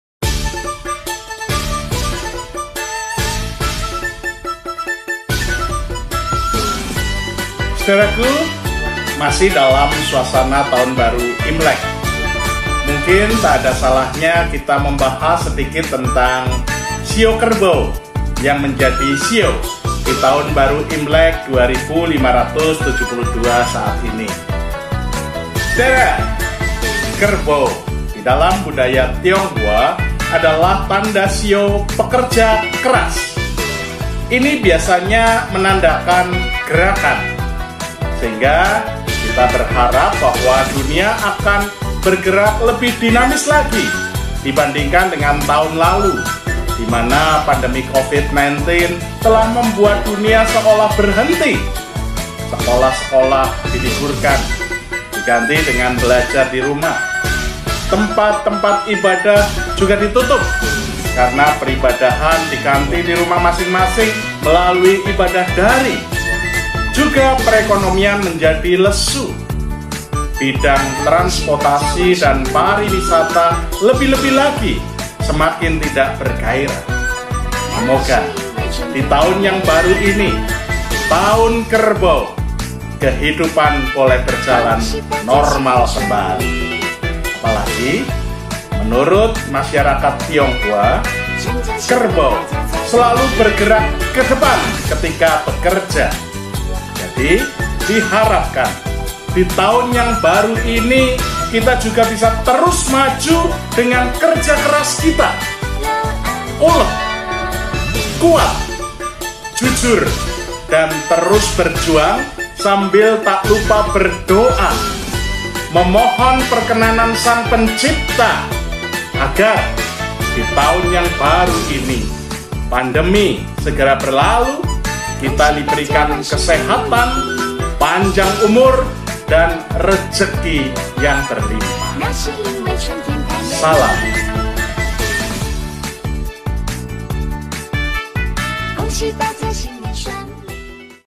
Setetes Embun Episode 42, "TAHUN KERBAU"
Tahun ini masyarakat Tionghoa memasuki tahun baru 2572, tahun kerbau. Kerbau adalah binatang pekerja keras, terus bergerak maju, ulet dan pantang menyerah. Dengan demikian diharapkan, kita semua boleh terus maju, terus berjuang, bekerja dengan keras, dan pantang menyerah untuk mencapai apa yang kita cita dan cintakan.